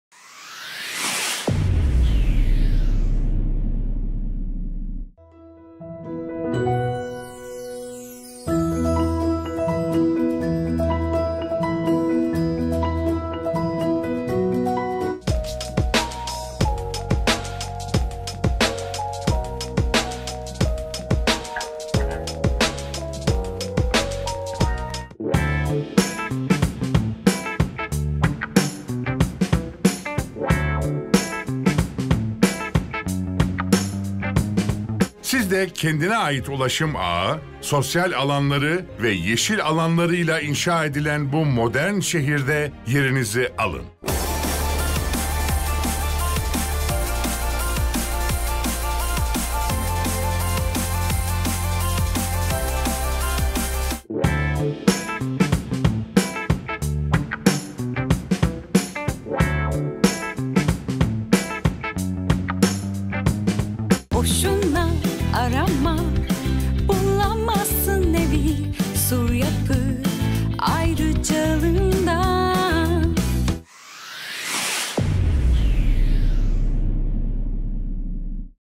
Sur Yapi TV (tr)
Teaser - is a 1 minute summary of branded TV channel, which broadcasts in points of sale (venues, sales departments, shops etc)